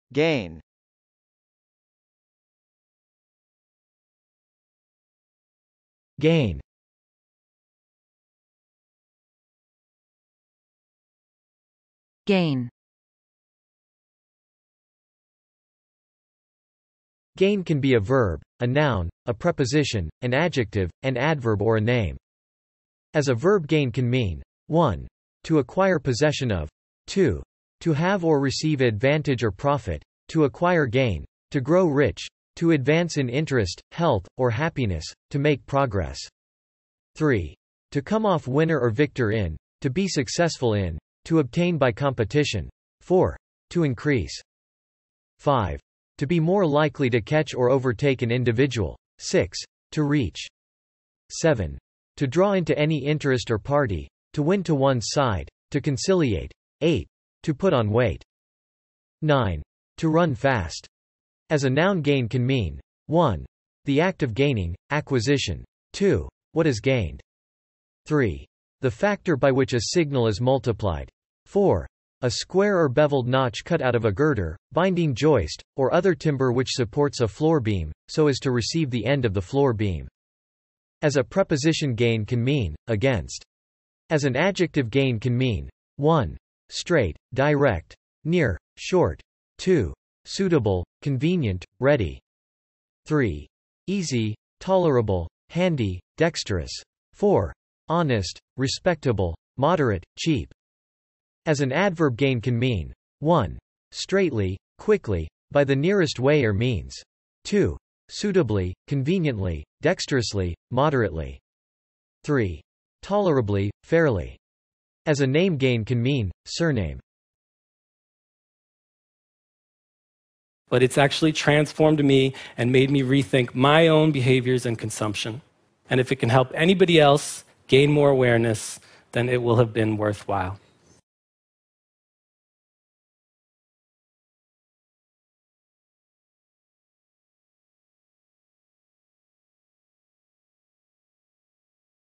GAIN - Meaning and Pronunciation
How to pronounce gain?

This video provides examples of American English pronunciations of gain by male and female speakers.
In addition, it explains the meaning of gain through a dictionary definition and several visual examples.

IPA Transcription of gain is /gˈeɪn/.

Definition of gain according to Wiktionary:
gain can be a verb, a noun, a preposition, an adjective, an adverb or a name

As a verb gain can mean:
1. To acquire possession of.
2. To have or receive advantage or profit; to acquire gain; to grow rich; to advance in interest, health, or happiness; to make progress.
3. To come off winner or victor in; to be successful in; to obtain by competition.
4. To increase.
5. To be more likely to catch or overtake an individual.
6. To reach.
7. To draw into any interest or party; to win to one's side; to conciliate.
8. To put on weight.
9. To run fast.

As a noun gain can mean:
1. The act of gaining; acquisition.
2. What is gained.
3. The factor by which a signal is multiplied.
4. A square or bevelled notch cut out of a girder, binding joist, or other timber which supports a floor beam, so as to receive the end of the floor beam.

As a preposition gain can mean:
Against.
As an adjective gain can mean:
1. Straight, direct; near; short.
2. Suitable; convenient; ready.
3. Easy; tolerable; handy, dexterous.
4. Honest; respectable; moderate; cheap.

As an adverb gain can mean:
1. Straightly; quickly; by the nearest way or means.
2. Suitably; conveniently; dexterously; moderately.
3. Tolerably; fairly.

As a name gain can mean:
Surname.

If you'd like to test your pronunciation of gain, check out Accent Hero's real-time pronunciation feedback tool: